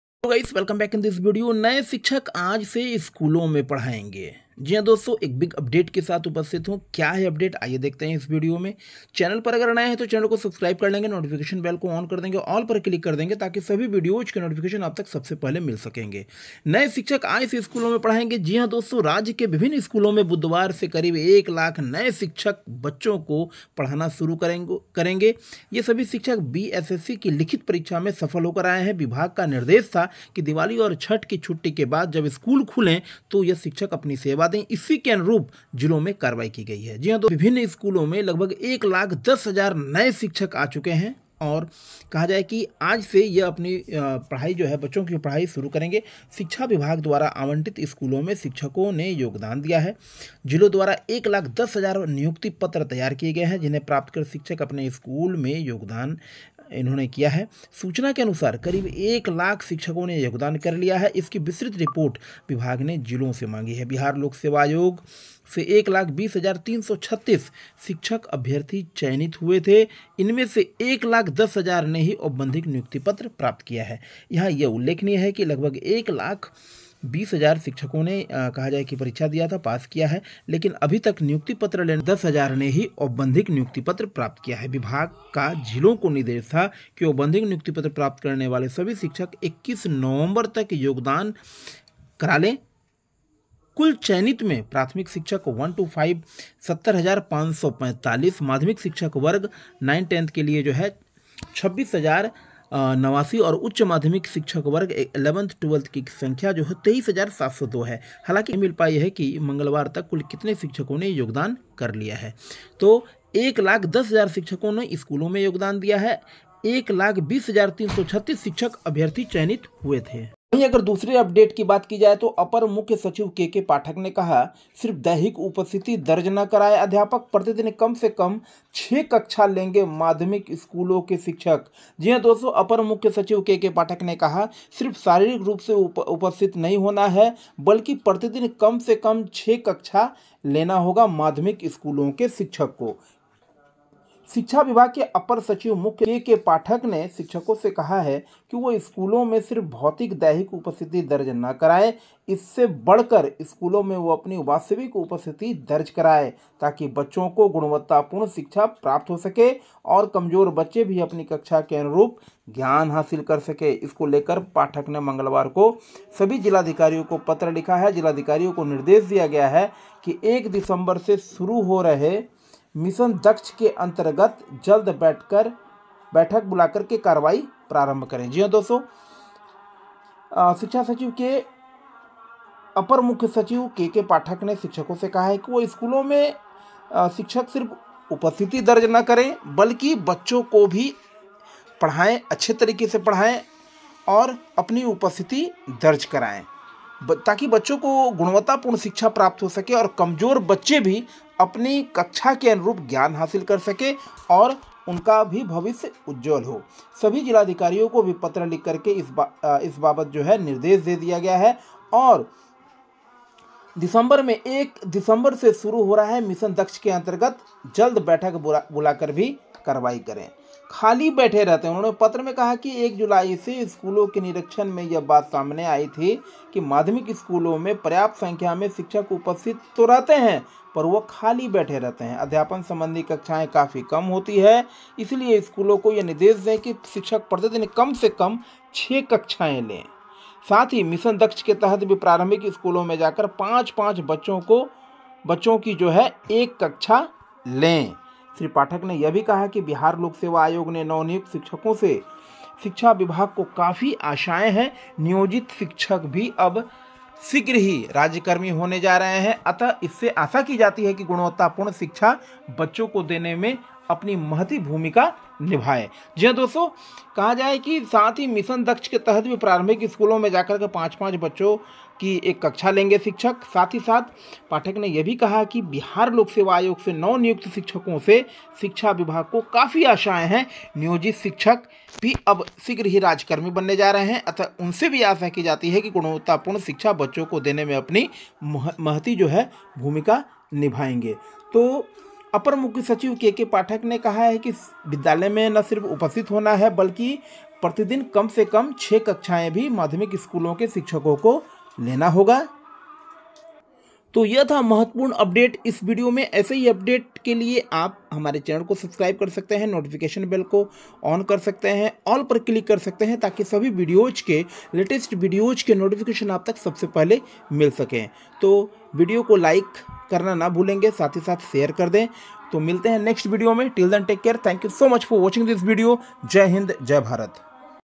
Bihar Teacher|सिर्फ दैहिक उपस्थिति दर्ज न कराएं अध्यापक| शिक्षकप्रतिदिन कम-से-कम छह कक्षा लेंगे|पाठक
Bihar Teacher Update |सिर्फ दैहिक उपस्थिति दर्ज न कराएं अध्यापक|माध्यमिक स्कूलों के शिक्षक प्रतिदिन कम-से-कम छह कक्षा लेंगे |केके पाठक